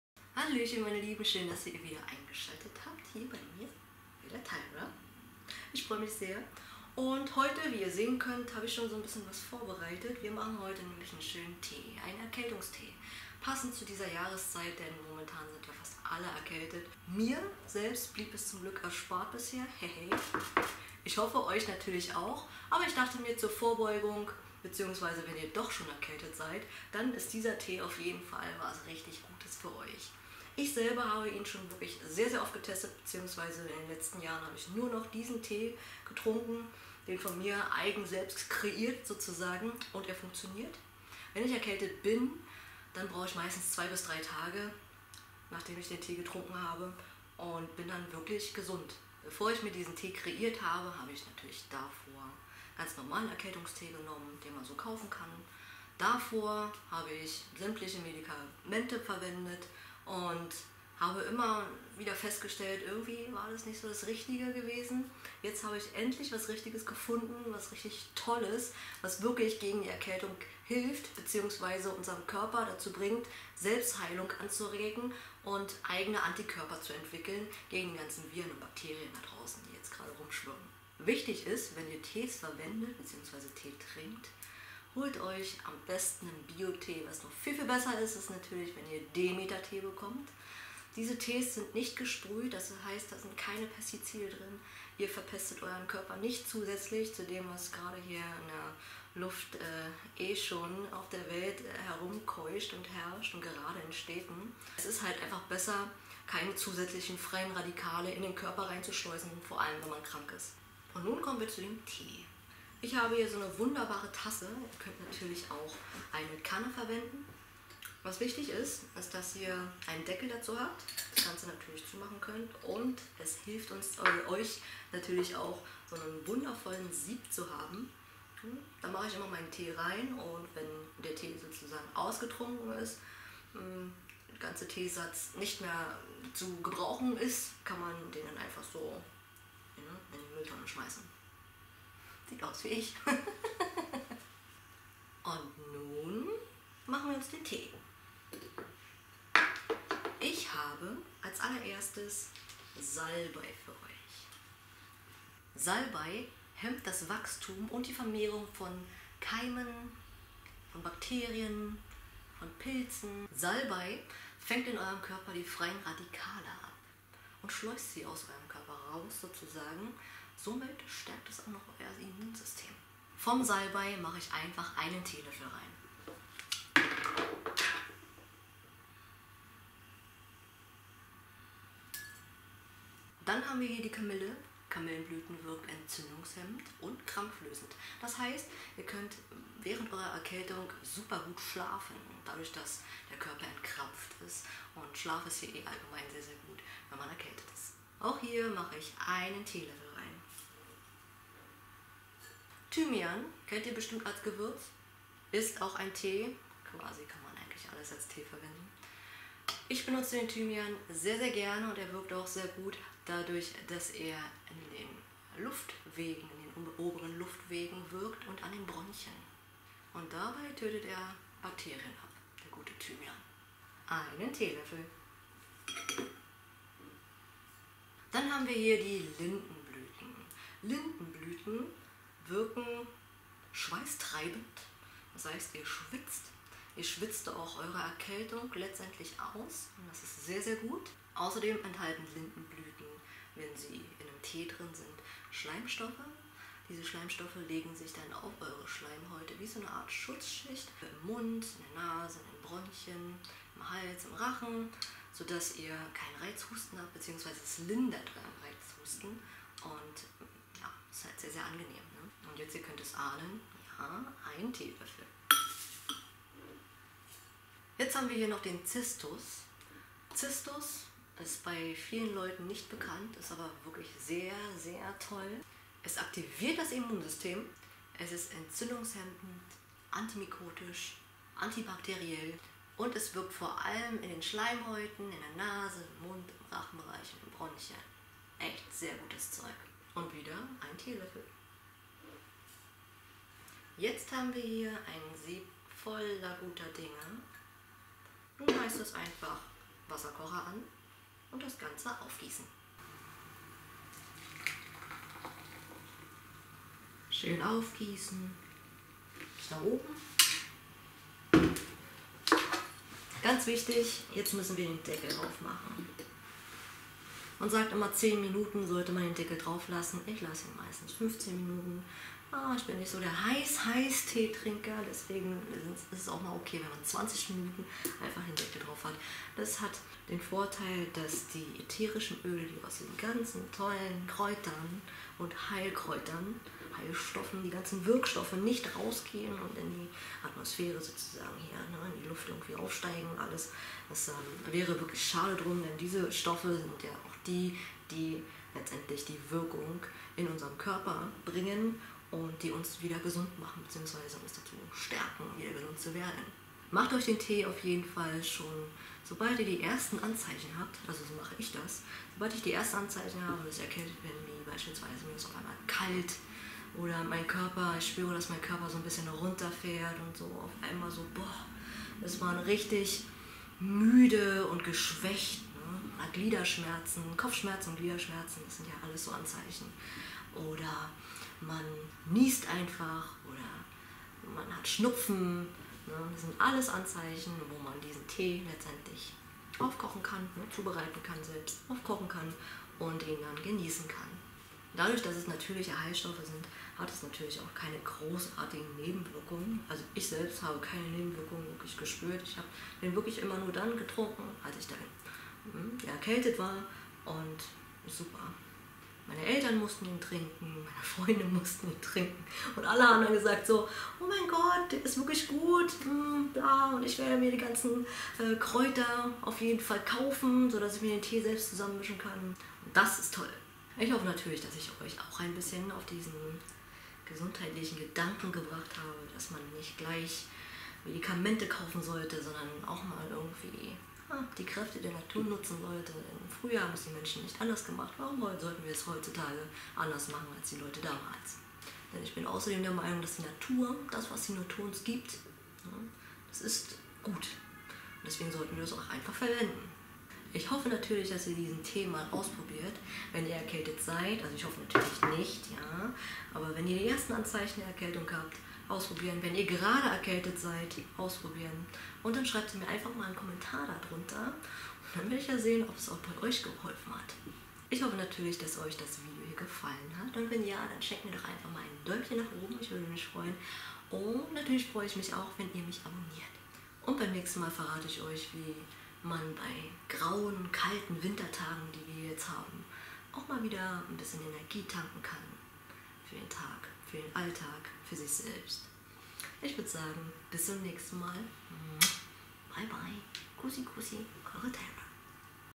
Erkältungstee selber machen | natürliche Heilung ohne Chemie
Gerade zur Herbst und Winterzeit, wenn das Wetter umschlägt und die kalte Zeit beginnt, leiden wir meist an Grippe und Erkältung. Eine wirklich schlimme Situation, doch müssen wir dann gleich zur Chemiekeule greifen oder geht das auch durch Heilmittel, die uns die Natur zur Verfügung stellt? Diese Frage habe ich mir schon vor vielen Jahren gestellt und bin der Meinung, dass man mit den richtigen Heilmitteln der Natur seine Erkältung schnell los wird ohne an einen Rückfall zu erleiden. In diesem Video stelle ich euch meine Erkältungs- Teemischung vor, mit der ich mich seit mehr als 3 Jahren heile, sobald ich an Grippe oder Erkältung leide.